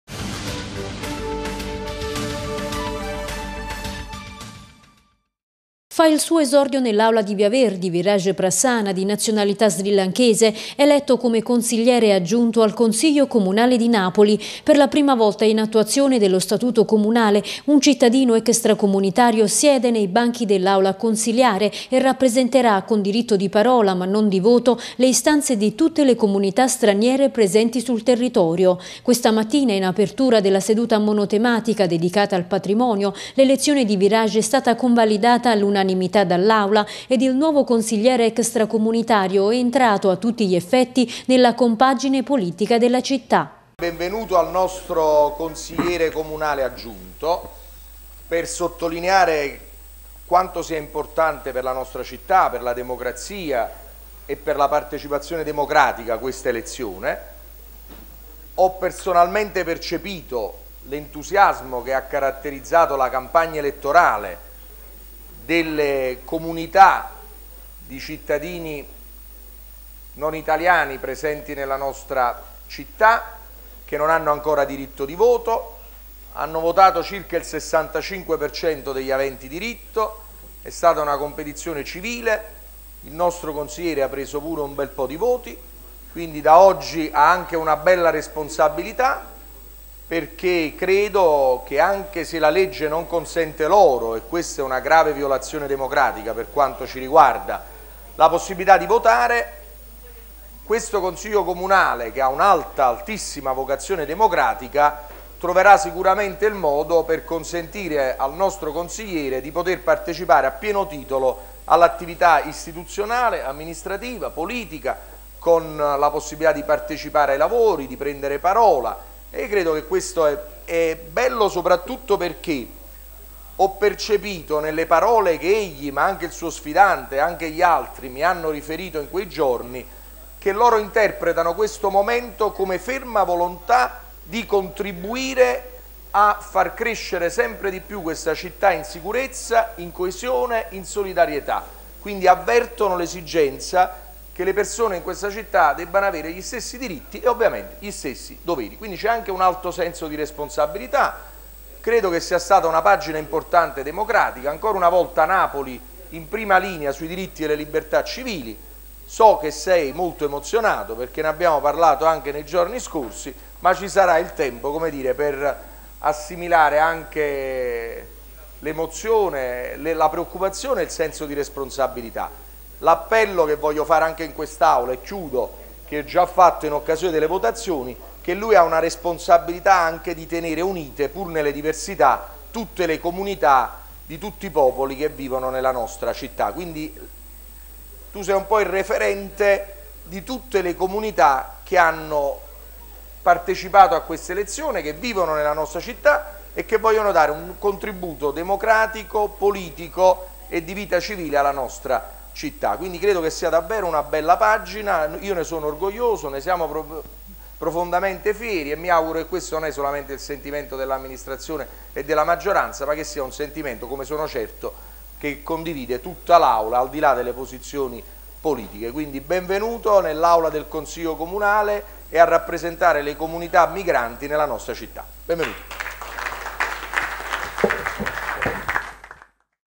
L'aula accoglie Viraj, nuovo consigliere extracomunitario
In apertura della seduta consiliare sul patrimonio, l'aula approva all'unanimità l'elezione di Viraj Prassana a consigliere aggiunto del Consiglio comunale di Napoli.